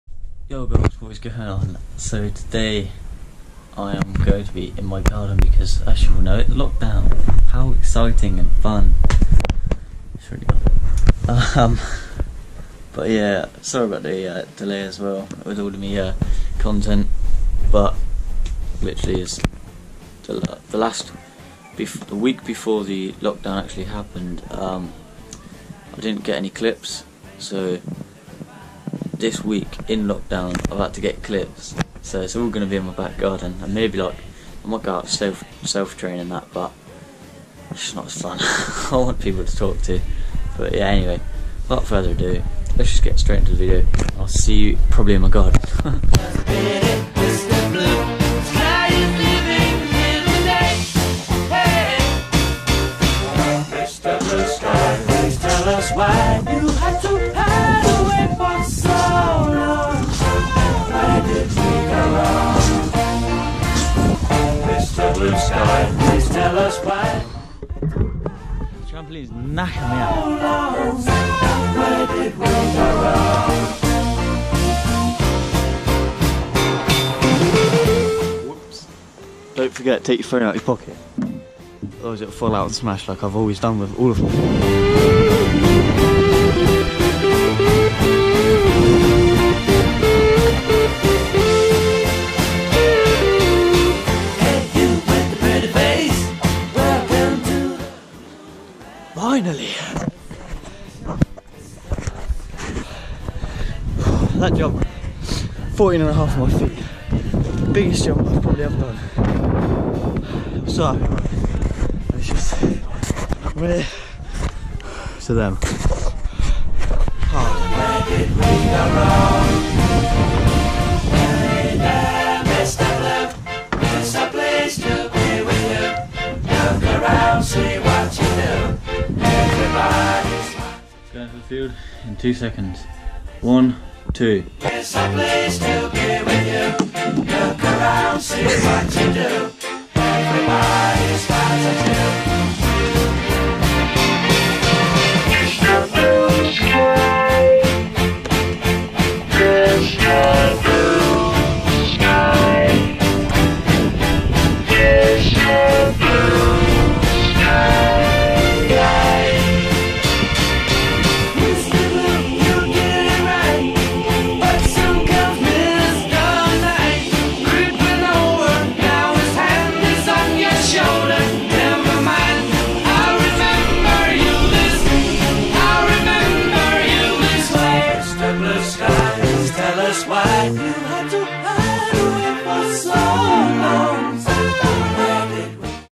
Getting my Parkour "Locked Down!!"
Sorry for re-uploading had to fix the audio. As most of you know its lockdown in the uk but this is what I have recorded I hope you enjoy!